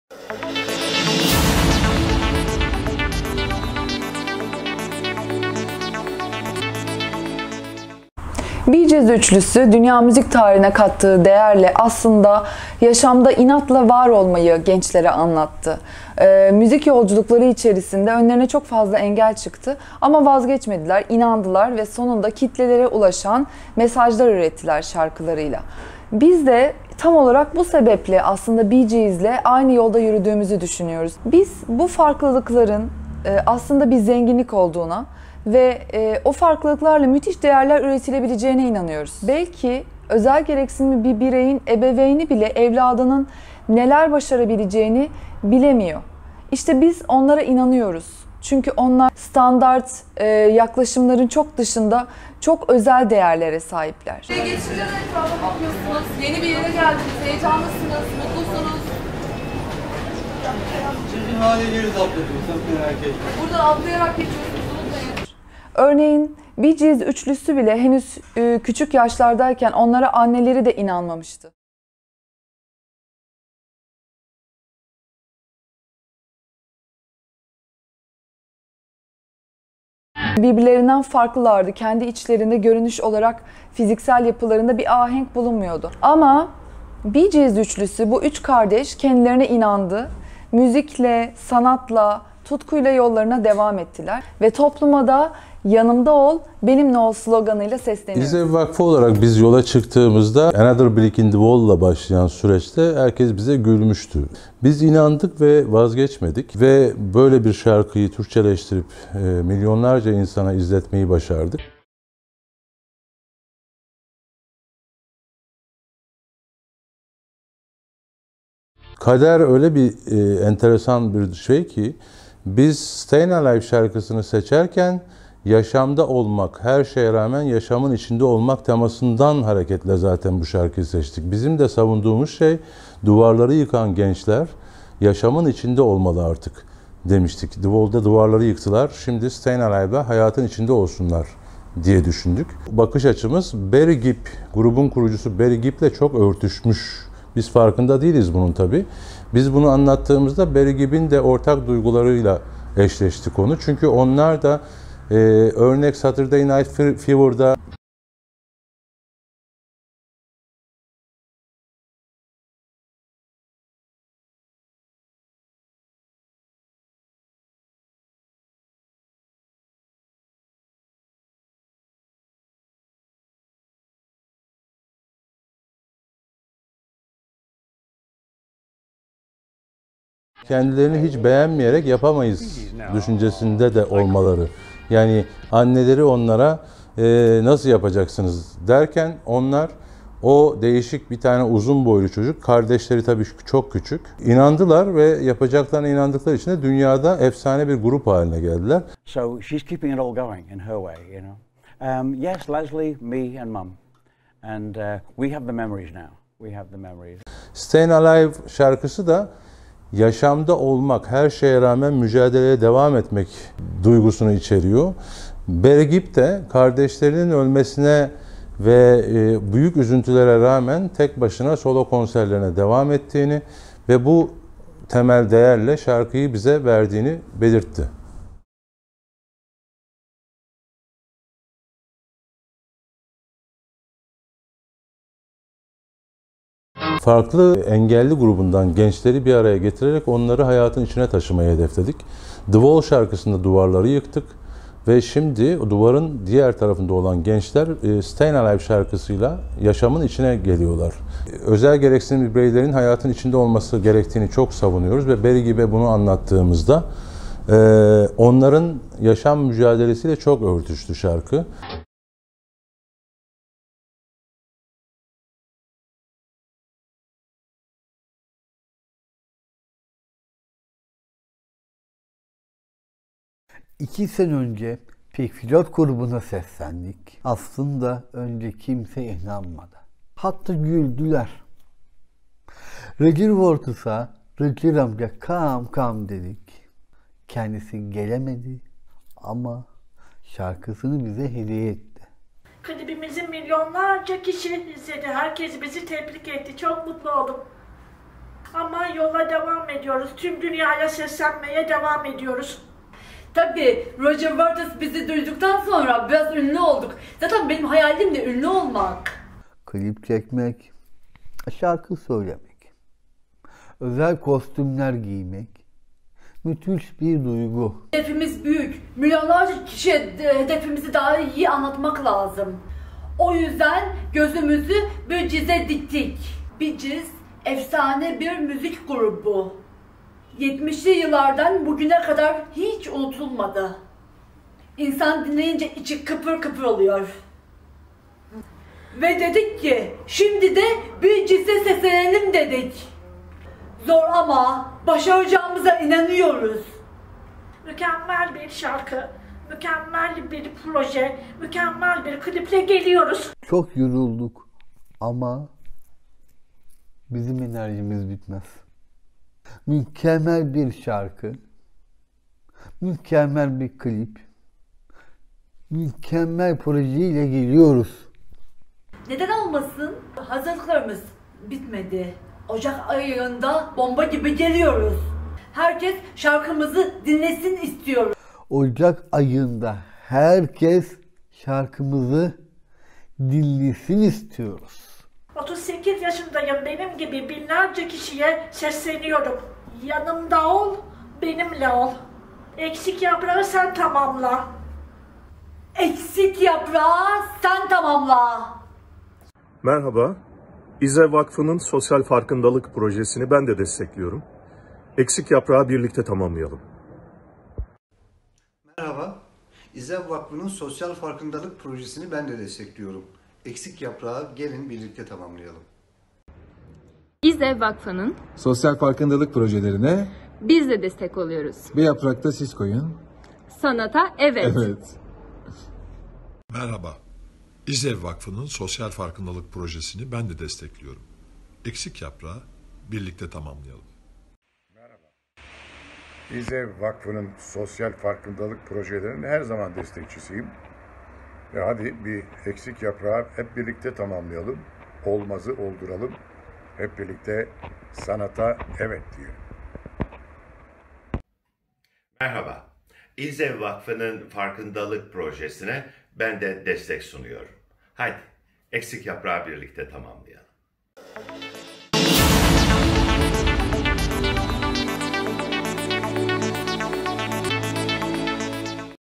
İZEV’in özel gereksinimli bireyler için yeni projesi dikkat çekiyor
3 Aralık Dünya Engelliler Gününde yeni sosyal sorumluluk projelerini kamuoyuyla paylaşan İstanbul Zihinsel Engelliler İçin Eğitim ve Dayanışma Vakfı (İZEV) son yıllarda engelli bireylerin hayatlarına dokunan farkındalık projeleriyle büyük ilgi görüyorlar.